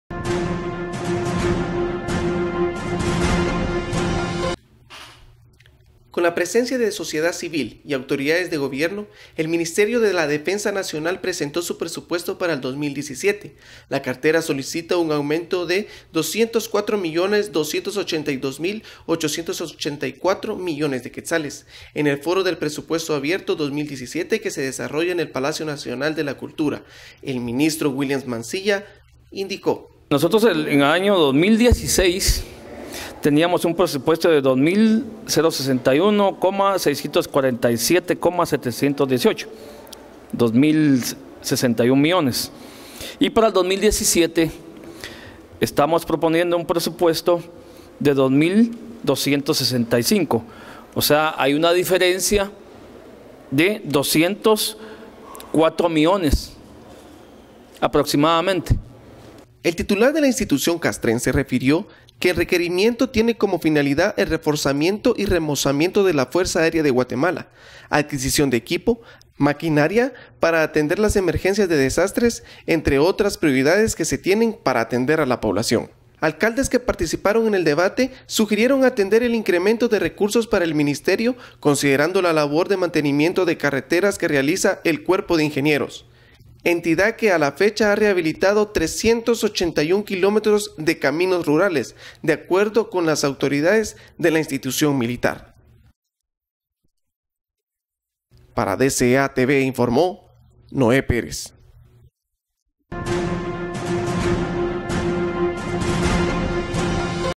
PRESUPUESTO 2017 DEL MINISTERIO DE LA DEFENSA NACIONAL
POR: NOE PEREZ-DCA
El titular de la institución castrense, refirió que el requerimiento tiene como finalidad el reforzamiento y remozamiento de la Fuerza Aérea Guatemalteca, adquisición de equipo, reparación de aeronaves para atender emergencias de desastres, entre otros prioridades que se tiene para atender a la población.
El Cuerpo de Ingenieros, entidad que a la fecha ha rehabilitado 381 kilómetros de caminos rurales.